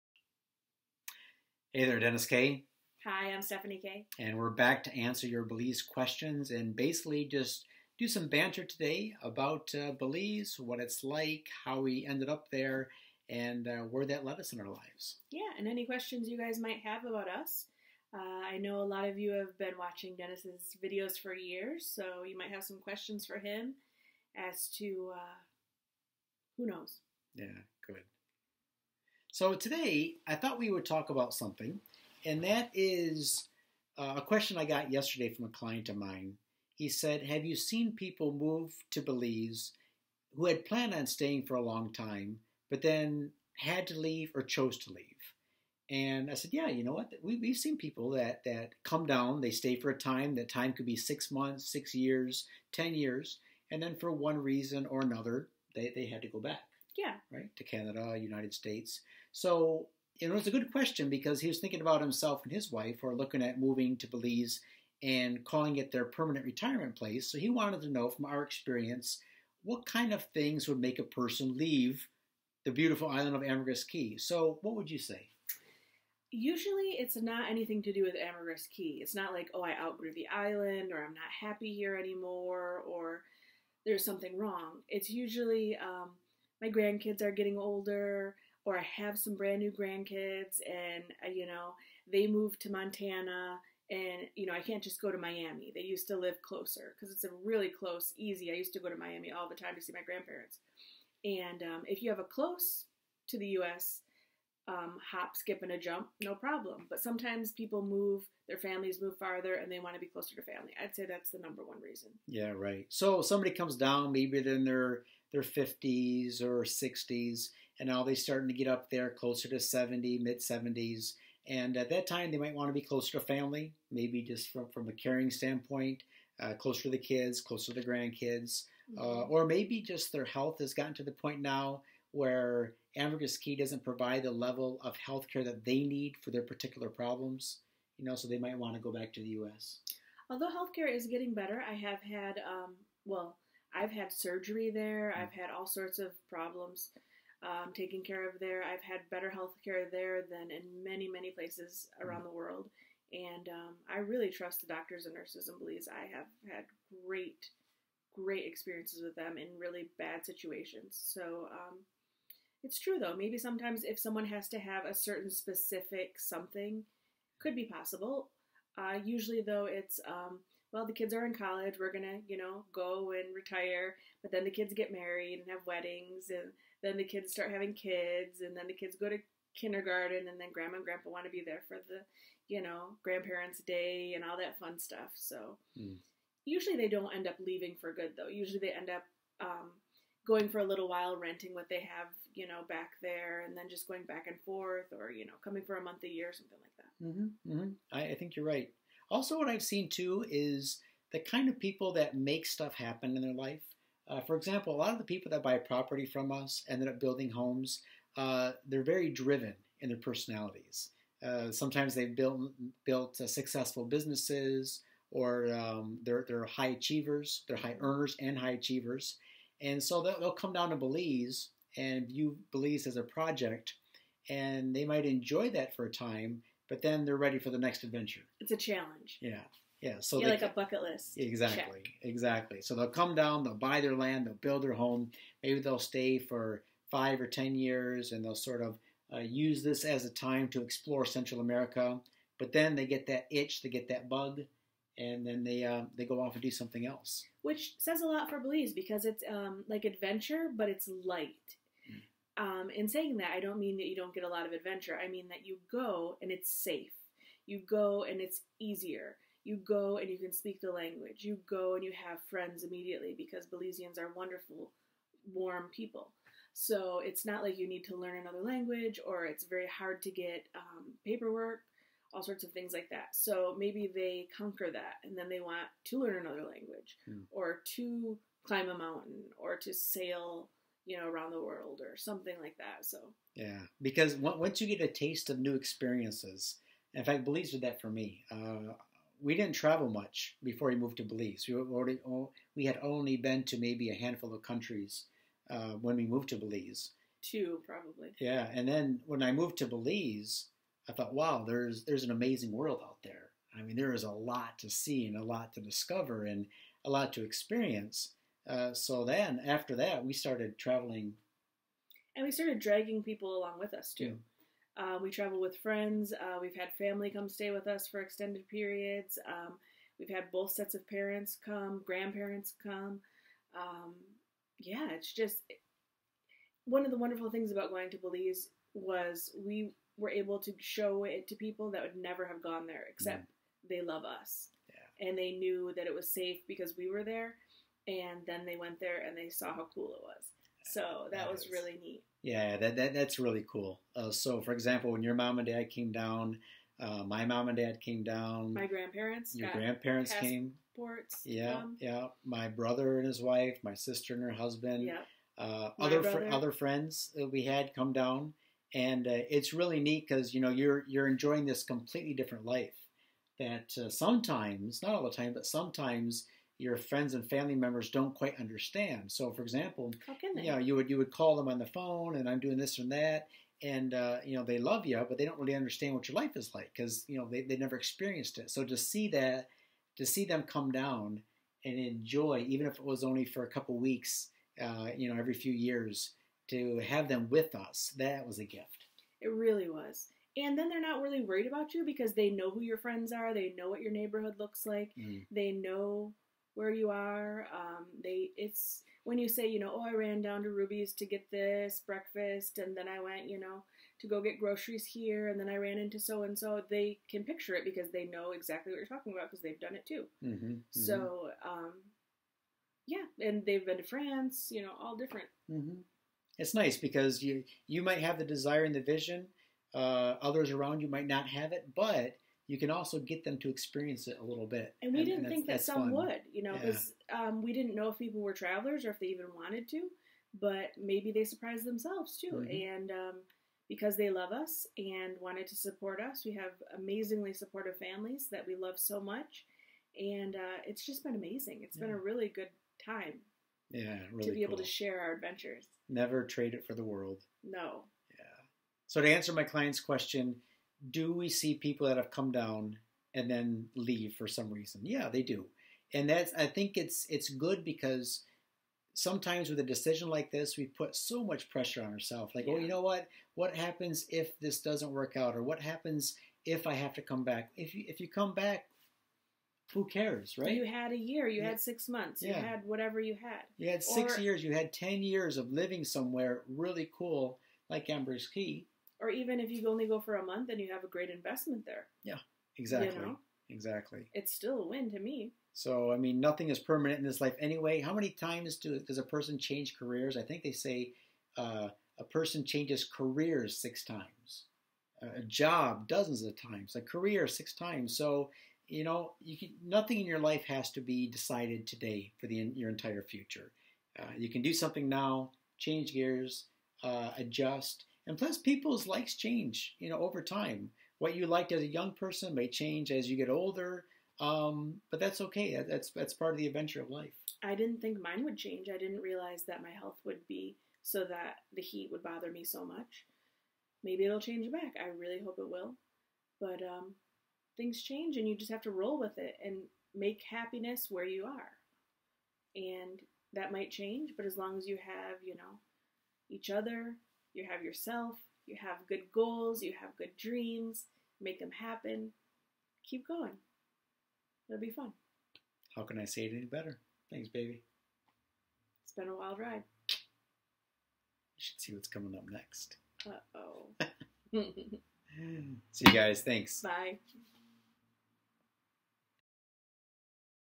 Expats in Belize - Is the Move Permanent or Temporary?
The average person will change careers 5-7 times during their working life according to career change statistics.

With an ever increasing number of career choices, 30% of the workforce will now change careers or jobs every 12 months.

What does this have to do with Belize?

Belize can be a permanent move. Or it can be a part time, temporary stint. A chapter or two in your life to mix things up a bit. Seek adventure. A break for the "normal".

For some people Belize is a permanent relocation. But for a growing number of people it's just the first step in a lifetime of adventure.

Dennis and Stephanie Kay
Belize Real Estate / Ambergris Caye Real Estate / Secret Beach Real Estate